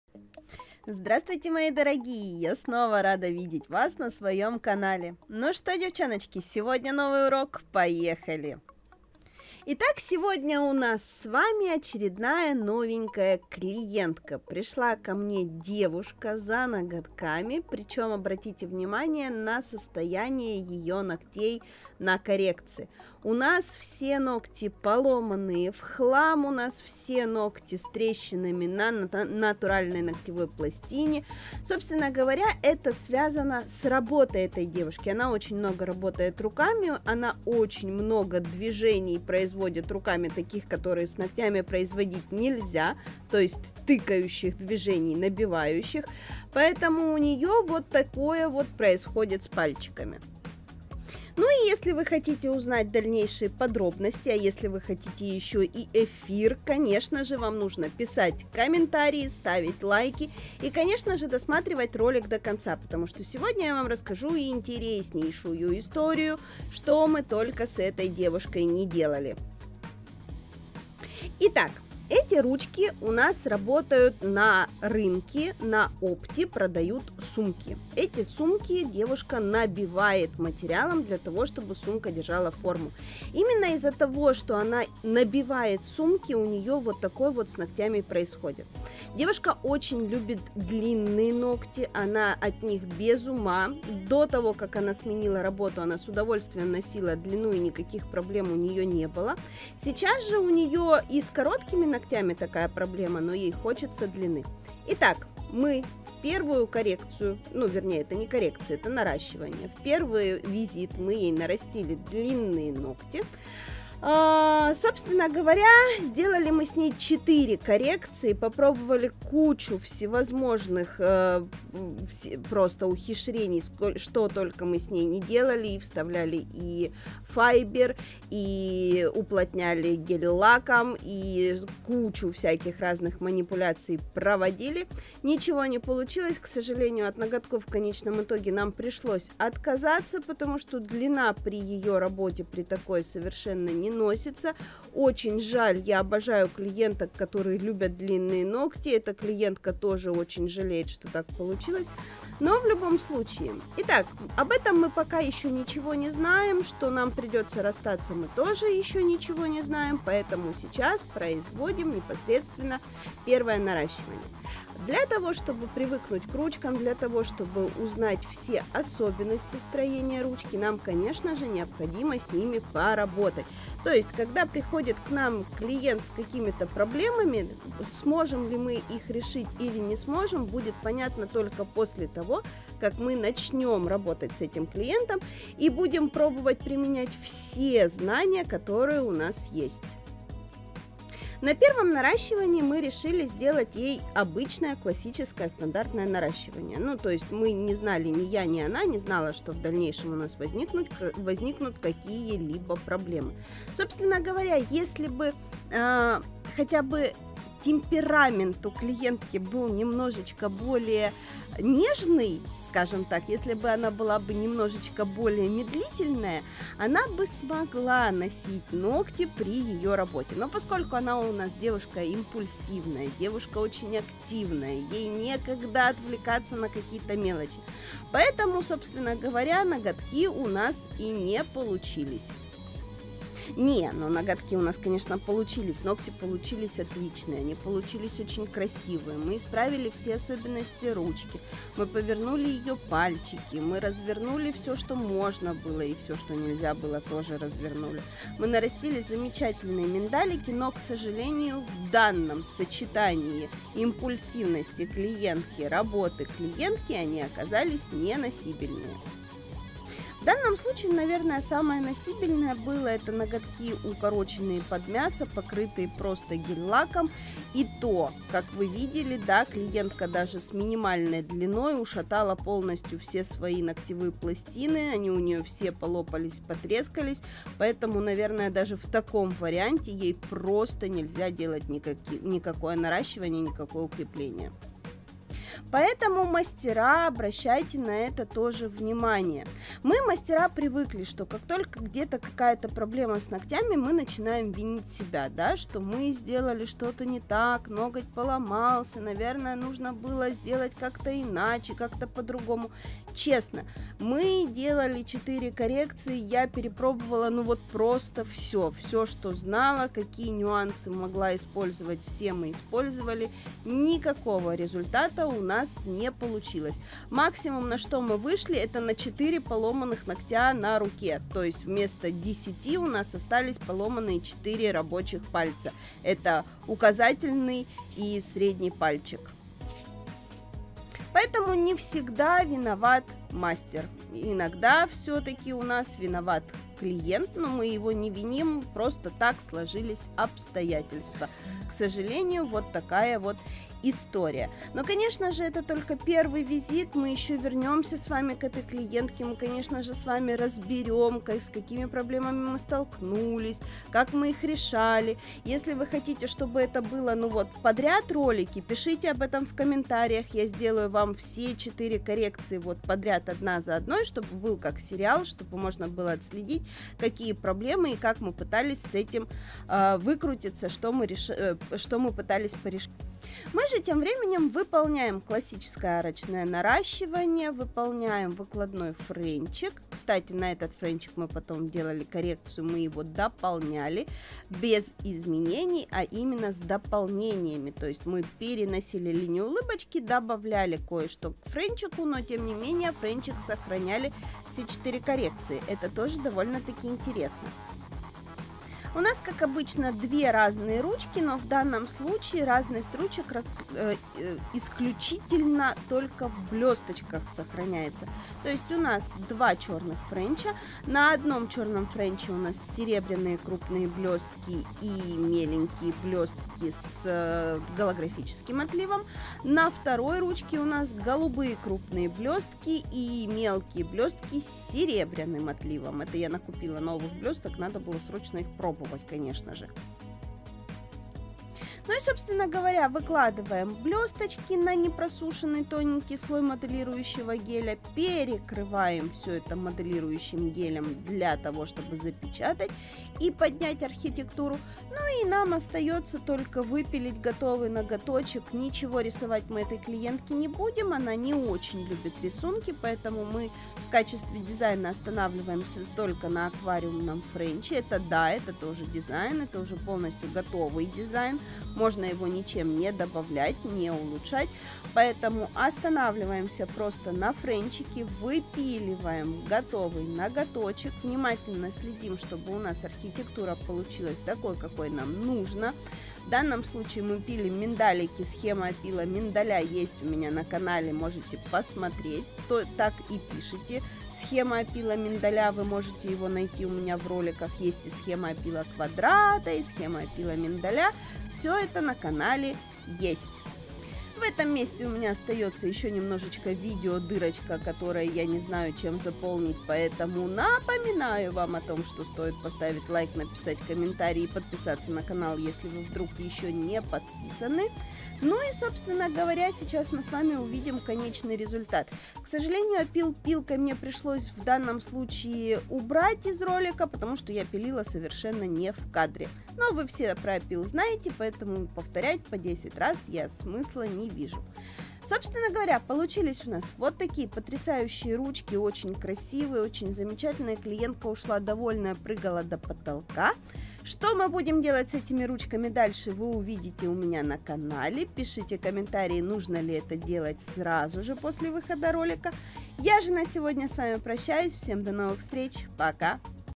новая клиентка. И Я НЕ СМОГЛА ПОМОЧЬ( сложности наращивания ногтей.
в наращивании ногтей встречаются такие сложности.. с которыми не возможно справится. вот в этот  раз я не смогла помочь новой клиентке

Буду рада видеть всех начинающих мастеров на онлайн курсах, на сайте представлен широкий ассортимент уроков:
Дизайн ногтей 2021, идеи маникюра